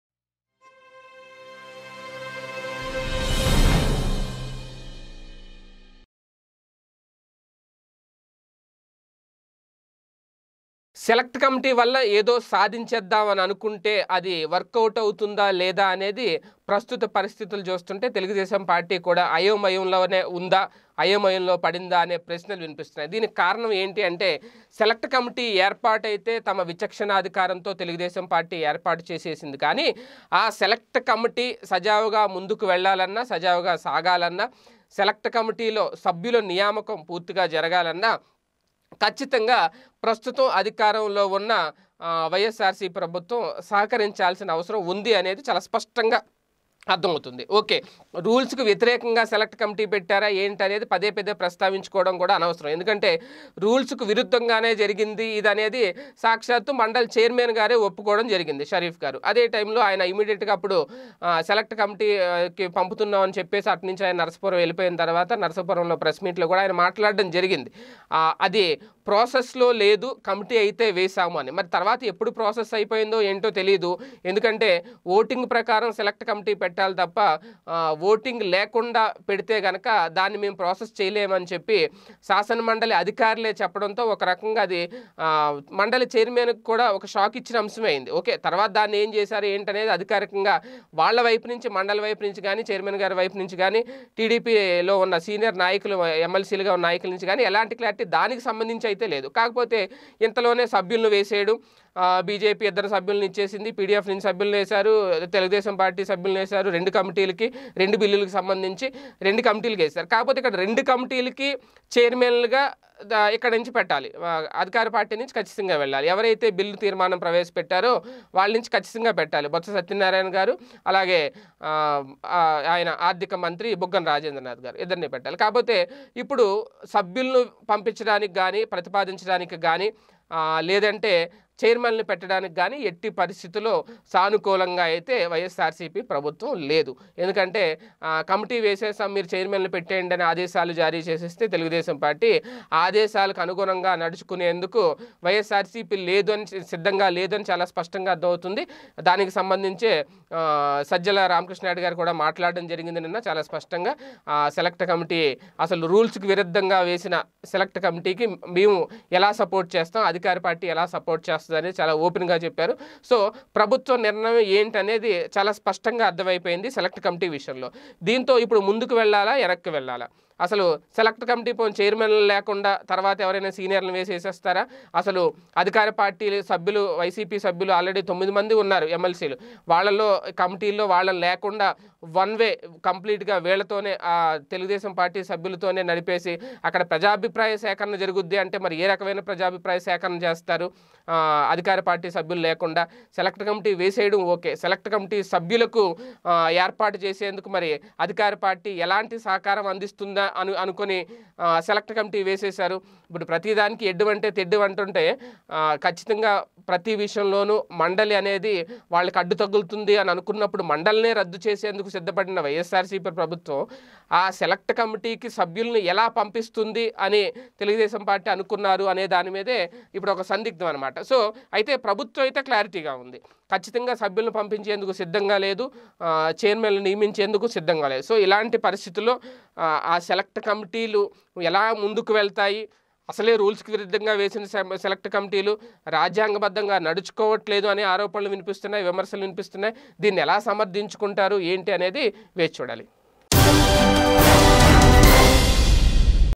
CM Jagan Mind Blowing Shock to Babu | సెలక్ట్ కమిటీకే పెద్ద షాకిచ్చిన జగన్.. చంద్రబాబు మైండ్ బ్లాక్
సెలక్ట్ కమిటీకే పెద్ద షాకిచ్చిన జగన్... చంద్రబాబు మైండ్ బ్లాక్ | CM Jagan Mind Blowing Shock to Chandrababu | PDTV News